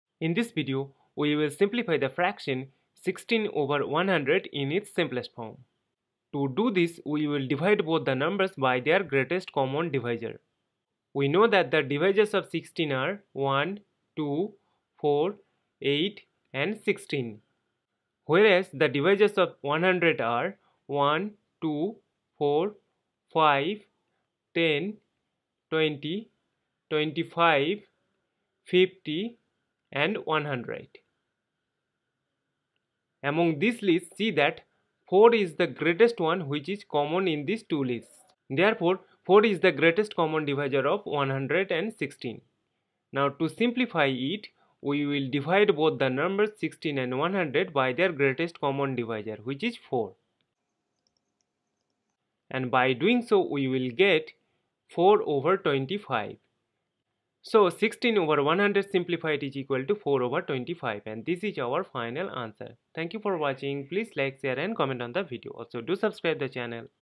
How to Simplify the Fraction 16/100 || 16/100 Simplified
Topic: How to simplify (reduce) the fraction 16/100 in its simplest form. Reduce 16/100 to lowest terms.

Queries Solved:
Q: What is 16/100 simplified?

Answer: 16/100 simplified is equal to 4/25.